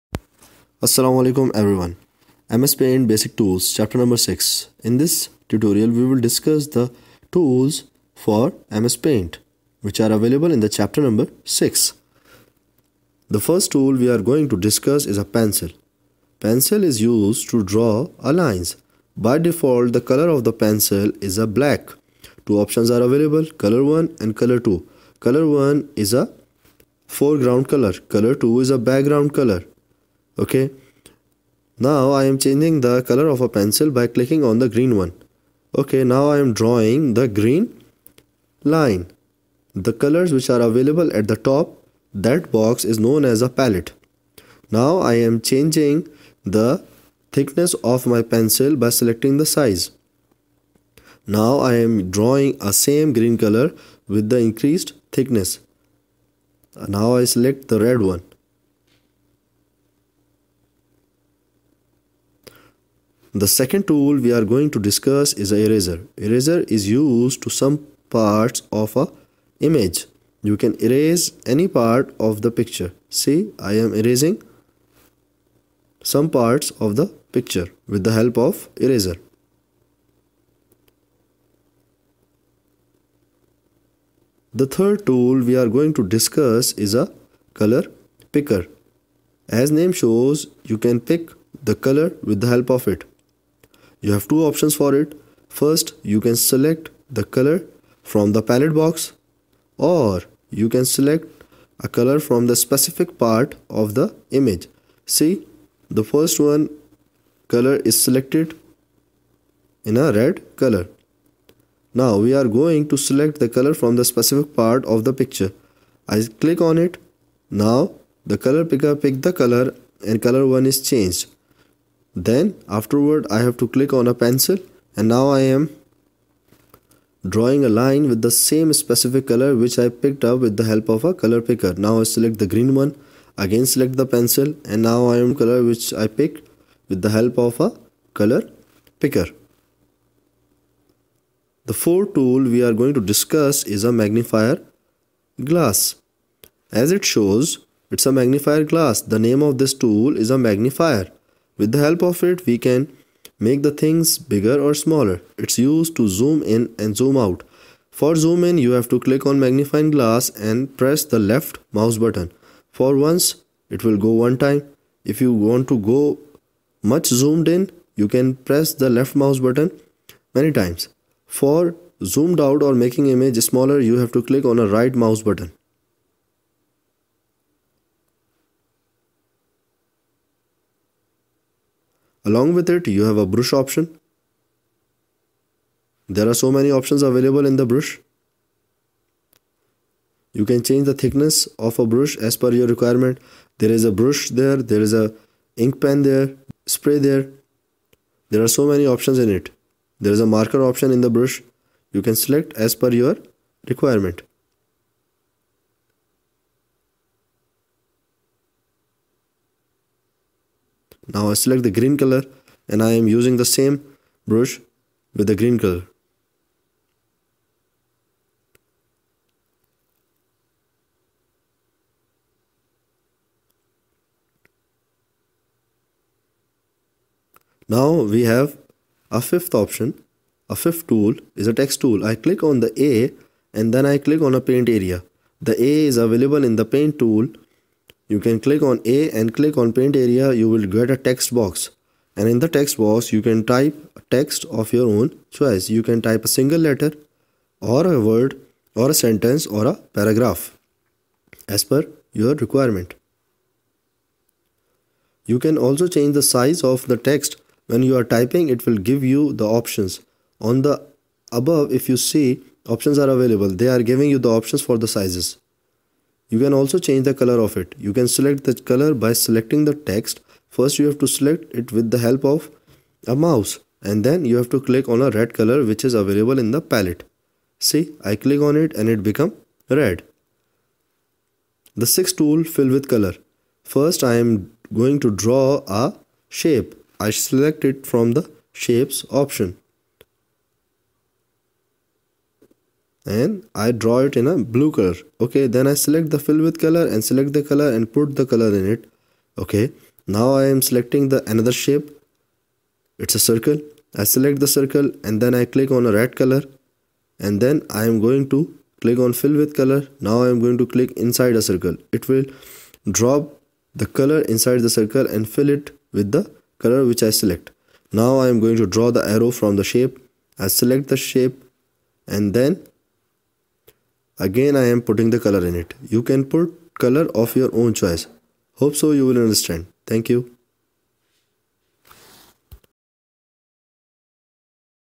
MS-Paint Tools | Chapter # 6 | Computer
This video is made for the tools of MS-Paint along with their working